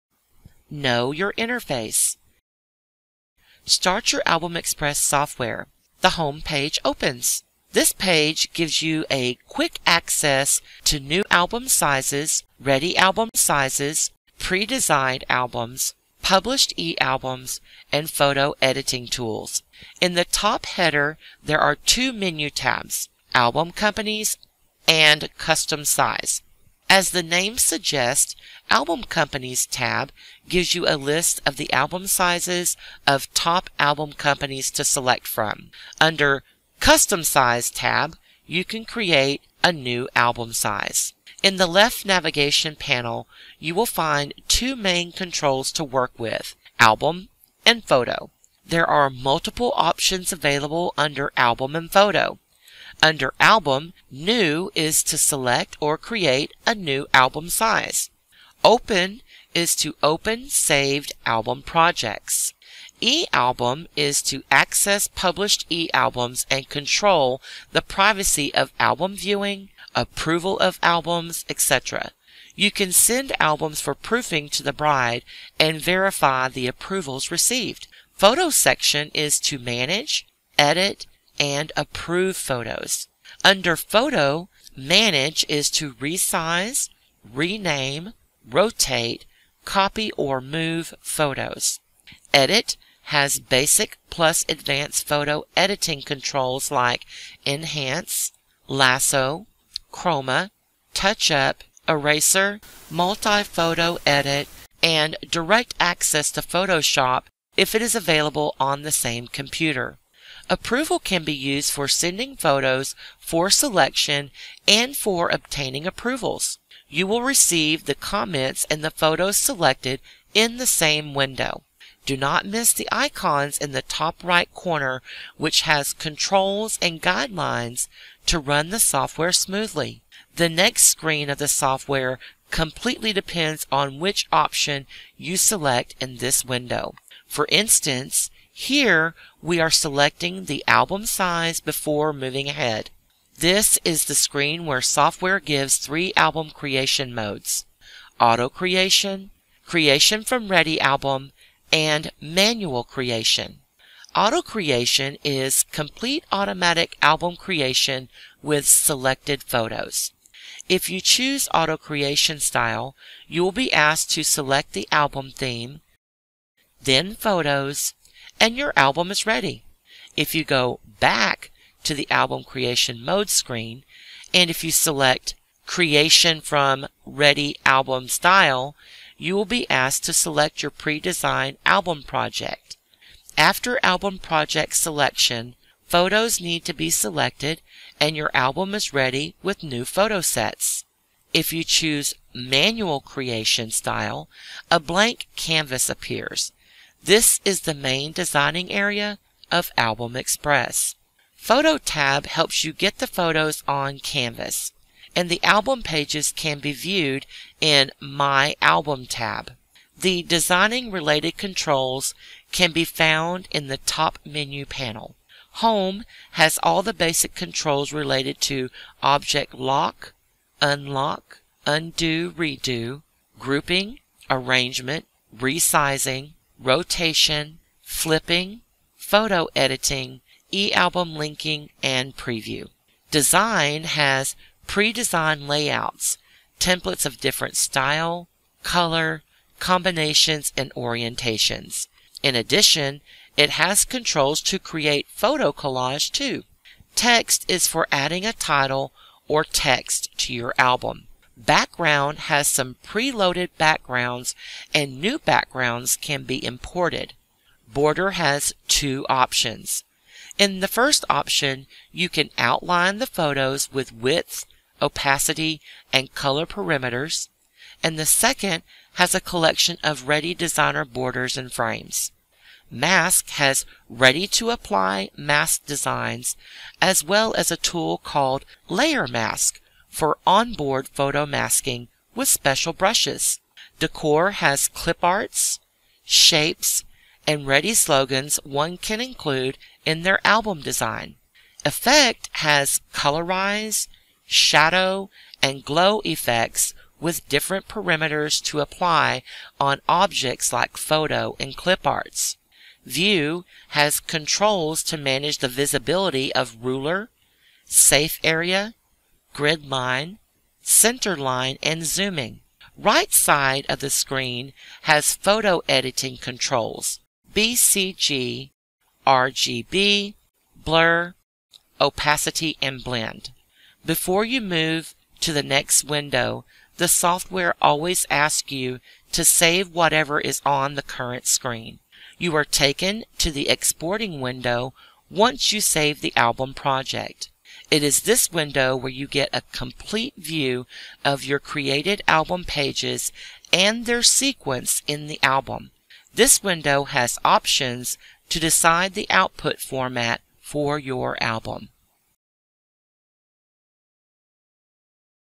AX_2 Know Your Interface
Introduction to the interface, basic tools and their functionality of the software.